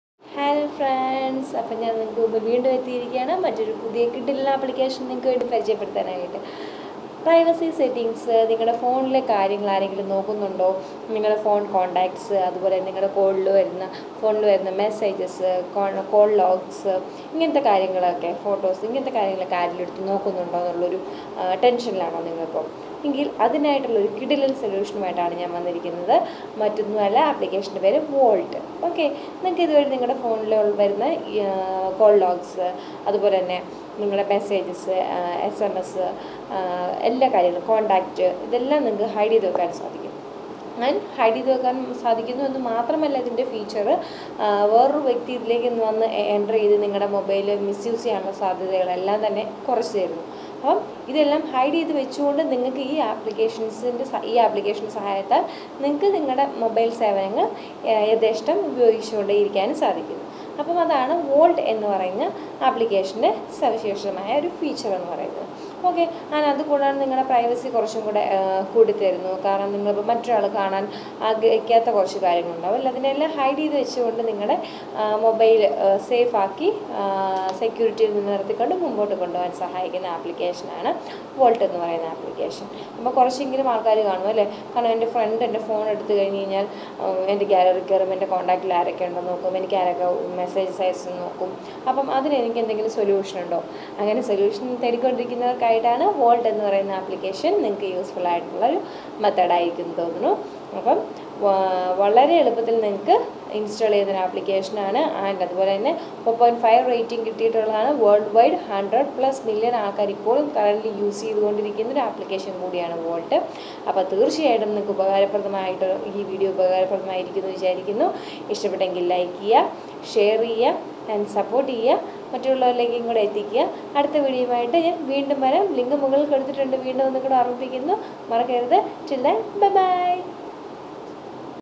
Hide Pictures Keep Safe Vault - Android App To Hide Photos
Hide Pictures Keep Safe Vault Android App is a well-rated app that millions of users use for keeping their private files away from the eyes of unauthorized persons.